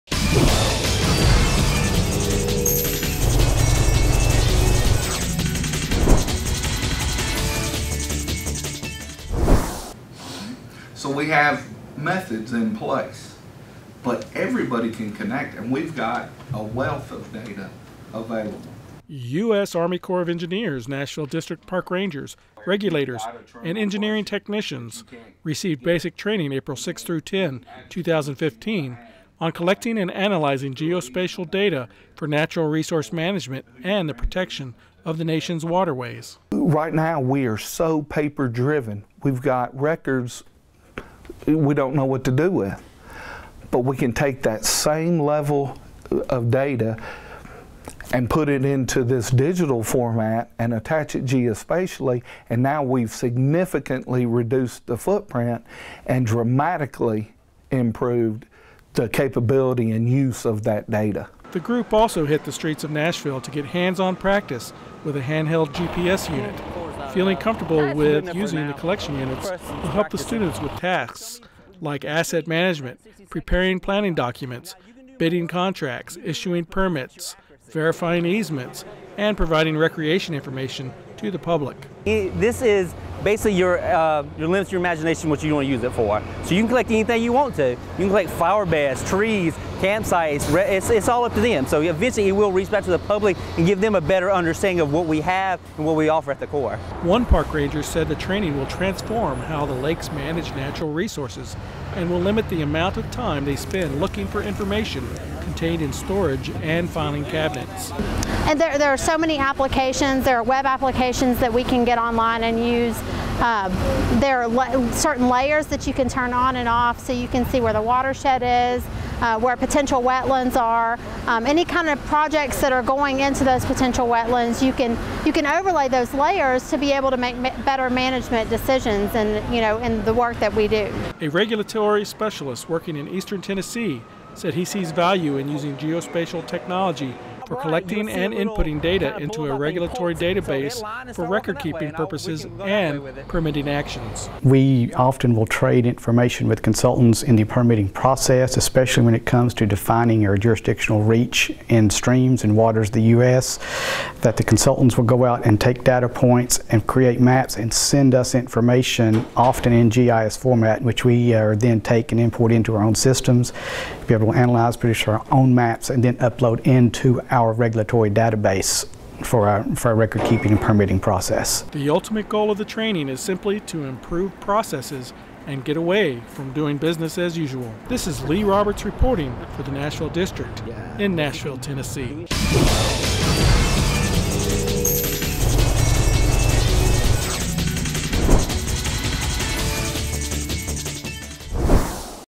Corps plots improvement with geospatial technology
The U.S. Army Corps of Engineers Nashville District is plotting to use geospatial data to improve its processes across the full spectrum of operations business lines by offering geospatial information systems basic training to its employees.  The technology is expected to reduce wasted time and effort searching for hard copy files contained in filing cabinets while increasing availability to digital data, which would be accessible online for both internal and public use. (USACE video by Lee Roberts)